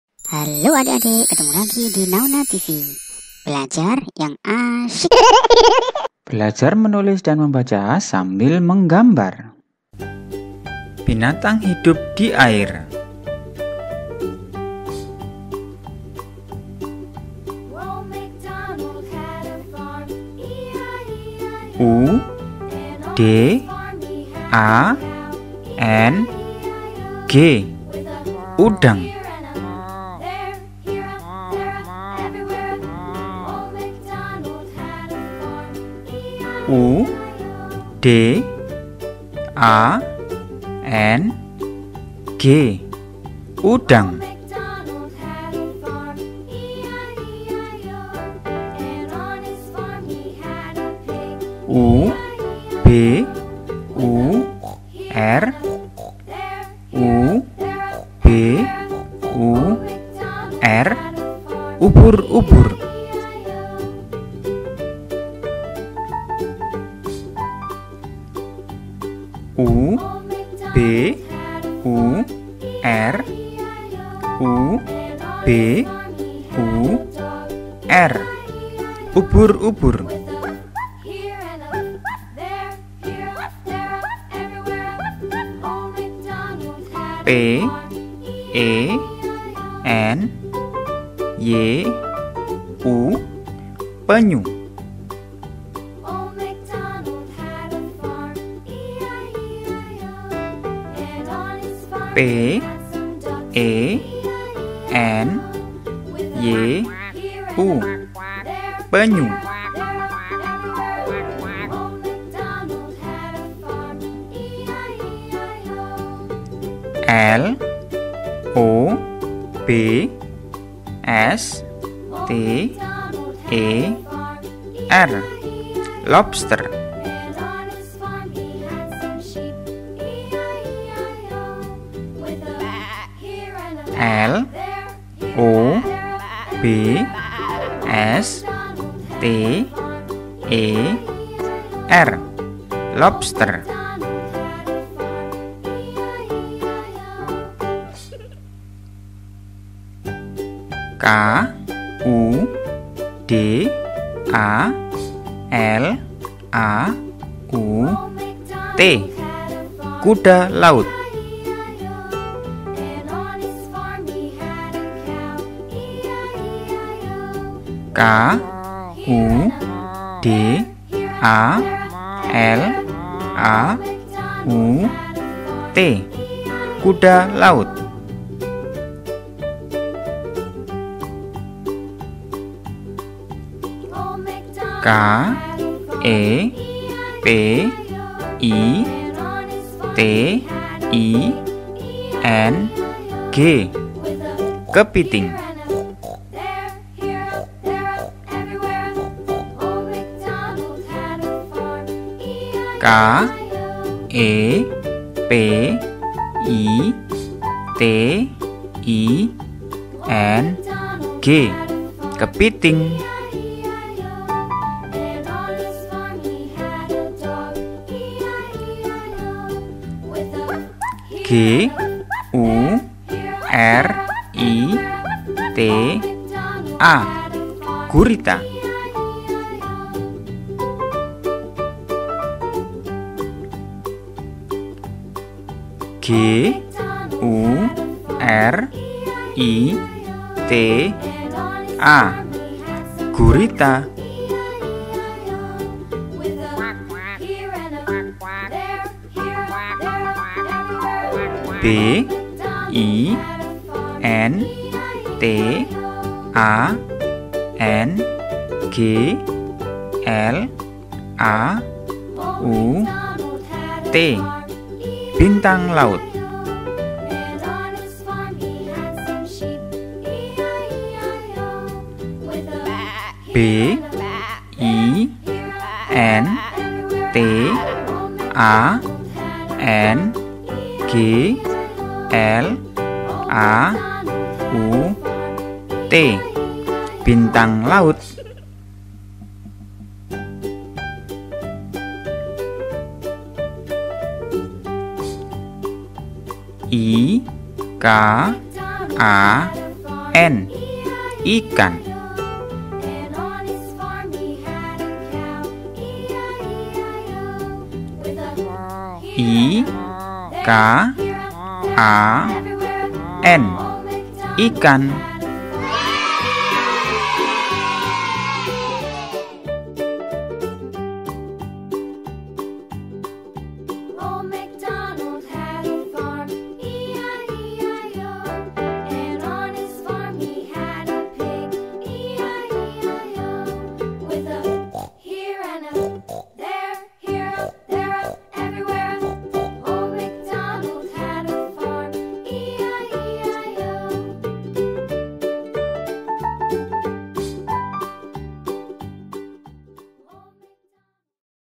belajar menulis, membaca dan menggambar Binatang HIDUP di AIR untuk anak PAUD TK SD
Belajar membaca
belajar berhitung